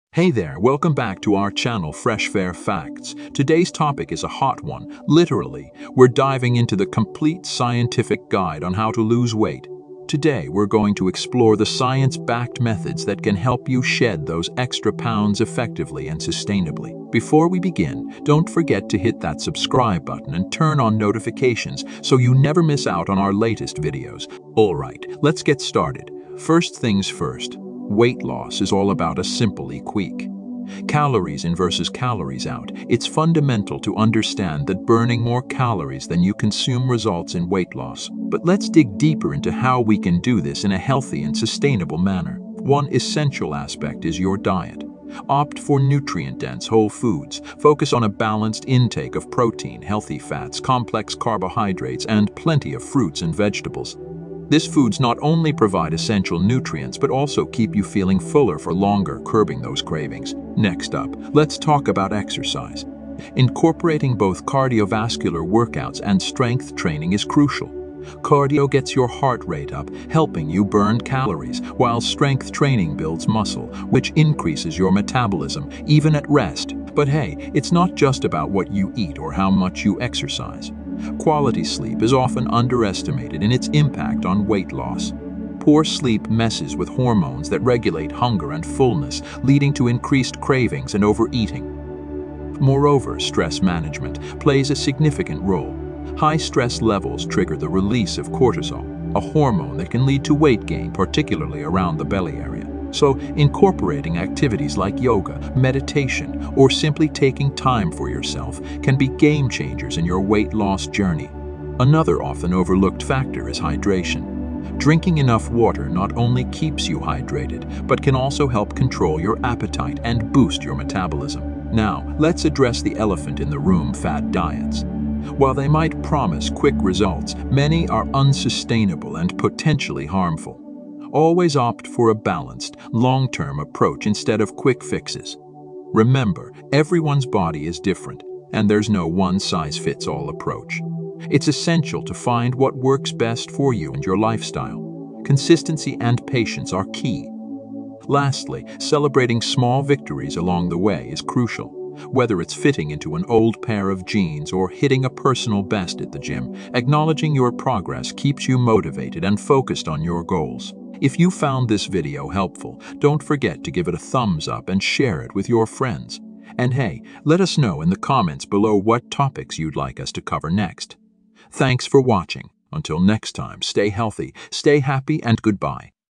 How to Lose Your body Weight & Become Fat to Fit in Proper Guide
In This Video we discover the science-backed secrets to successful weight loss in this comprehensive guide! Dive into proven strategies, from diet tips to exercise routines, sleep hacks, and stress management. Learn how to transform your lifestyle sustainably and achieve your weight loss goals effectively.

0:00 - Intro
0:04 - The Science of Weight Loss
0:25 - Scientific Information
0:42 - Balanced Diet
1:00 - Exercise Routine
1:17 - Good Sleep
1:31 - Stress And Cortisol Effects
1:51 - Hydration Importance
2:01 - Fad Diets And Balanced Approach
2:15 - Conclusion
2:40 - Outro